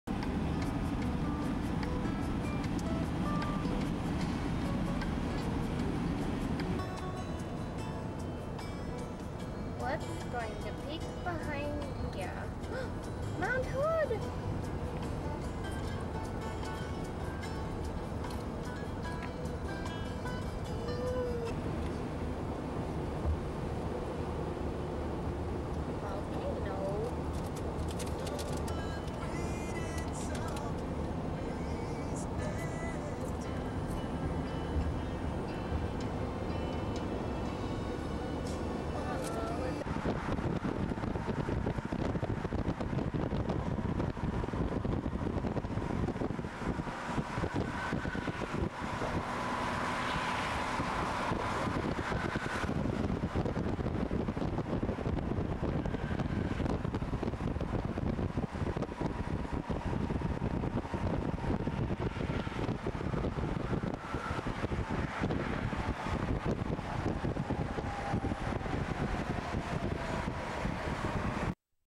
Mt Hood & Portland Skyline Oregon
Our trip by the famous volcano and beautiful city of roses...Portland, Oregon